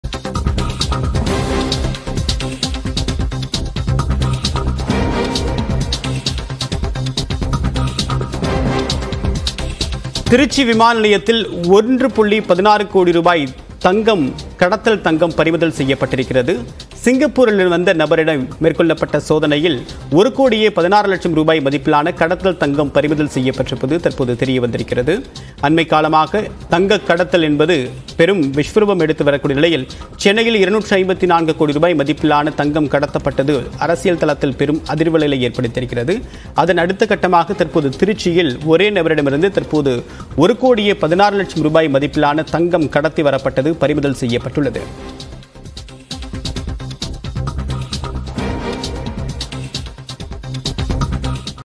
Trichy Airport | ரூ.1.16 கோடி கடத்தல் தங்கம் பறிமுதல் - எங்கு தெரியுமா? | Just Now
திருச்சி விமான நிலையத்தில் ரூ.1.16 கோடி மதிப்பிலான கடத்தல் தங்கம் பறிமுதல்

list=PLZjYaGp8v2I8Jl8PfHo2MQdWKs42-15cX